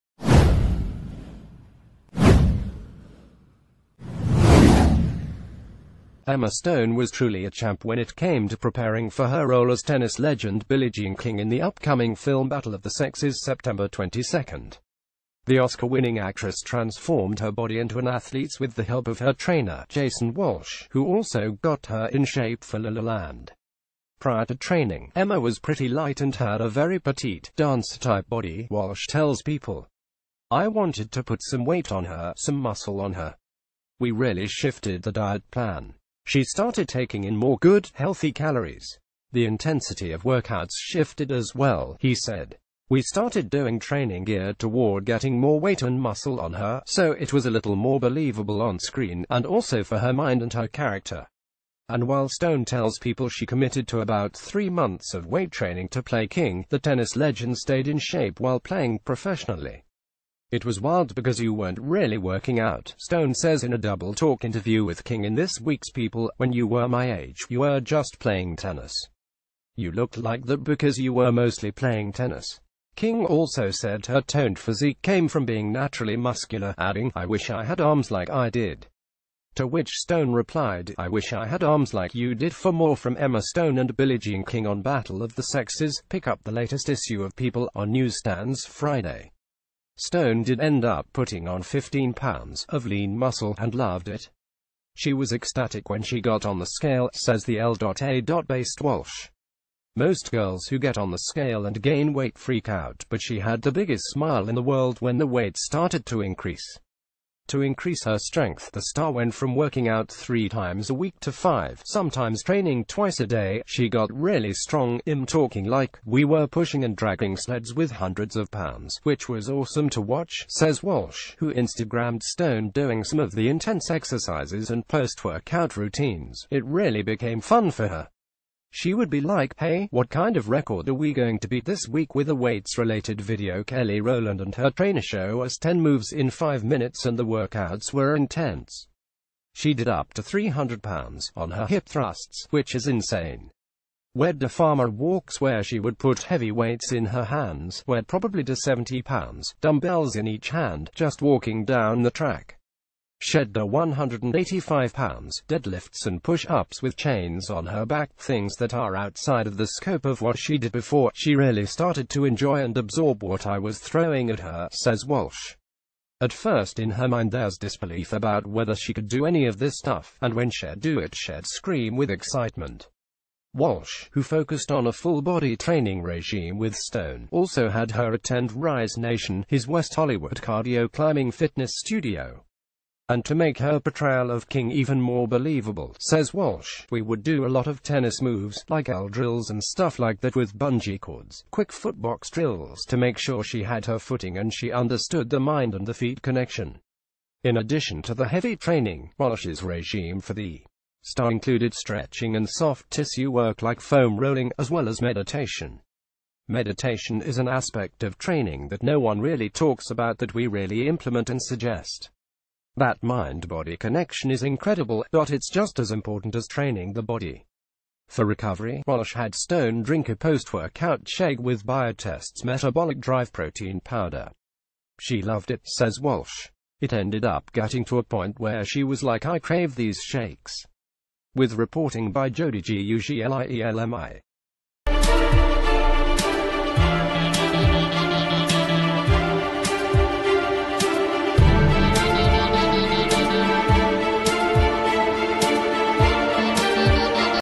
Breaking News | Emma stone was 'ecstatic' to gain 15 lbs. of muscle for battle of the es, says her
Emma stone was 'ecstatic' to gain 15 lbs. of muscle for battle of the es, says her trainer

Emma Stone was truly a champ when it came to preparing for her role as tennis legend Billie Jean Kin...